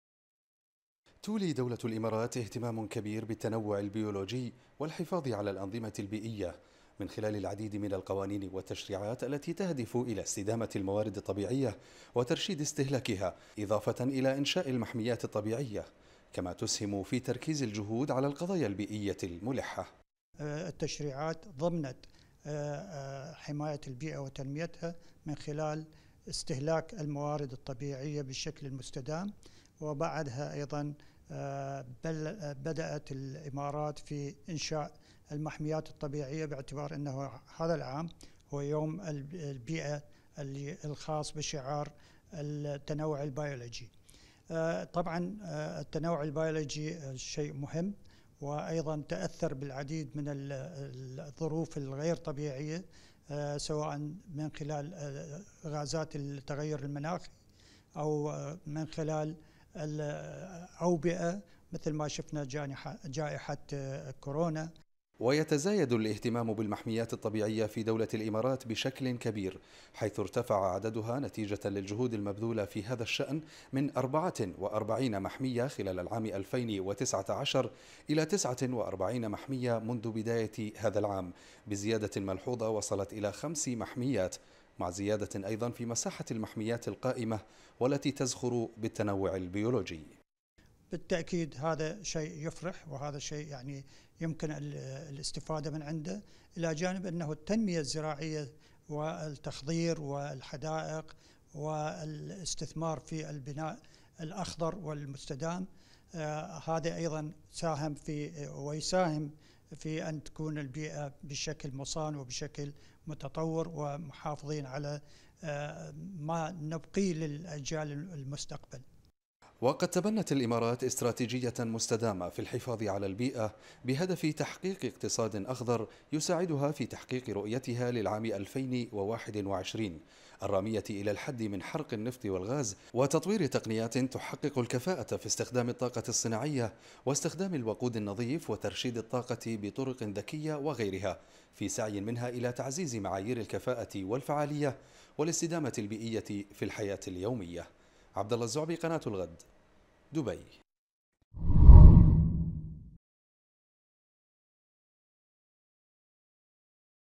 الإمارات تشارك في الاحتفالات باليوم العالمي للبيئة الموافق 5 يونيو
الإمارات - الغد: تشارك الإمارات دول العالم الاحتفال باليوم العالمي للبيئة الذي أقرته الأمم المتحدة في الخامس يونيو من كل عام لتعزيز وعي المجتمعات بأهمية حماية البيئة وضمان استدامة مواردها الطبيعية وتنوعها البيولوجي.
المزيد من التفاصيل في سياق تقرير مراسل الغد في الإمارات عبد الله عبر النشرة الإخبارية.